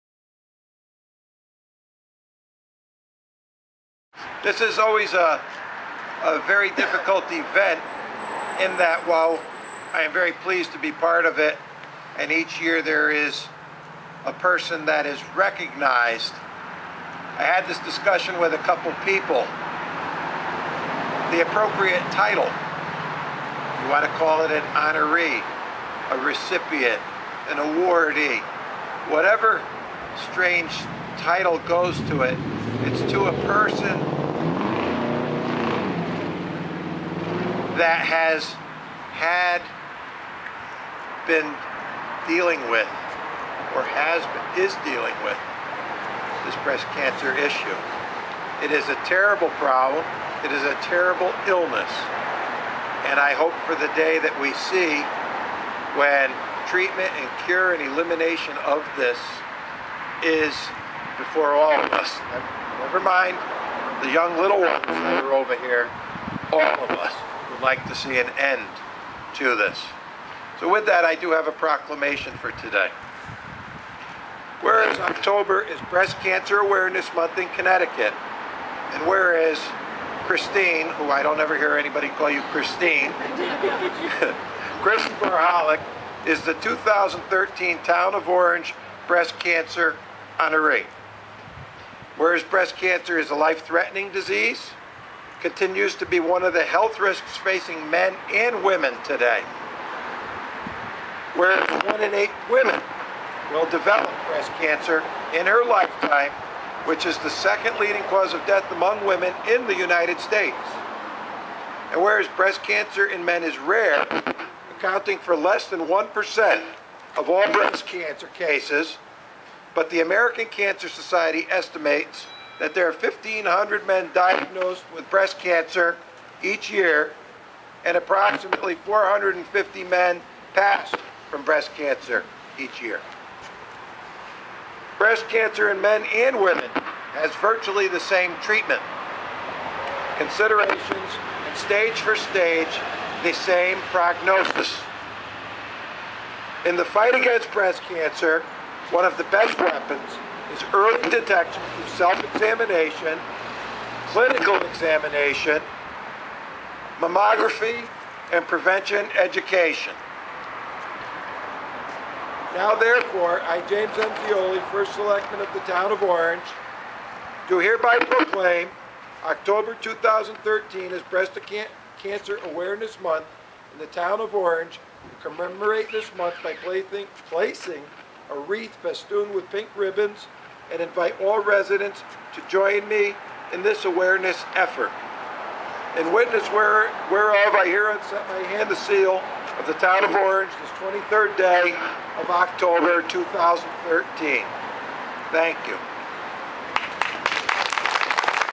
Cancer Wreath Zeoli Proclamation
First Selectman Jim Zeoli reads a proclamation during the Breast Cancer Awareness Month Ceremony at the Orange CT Town Hall on Oct. 23.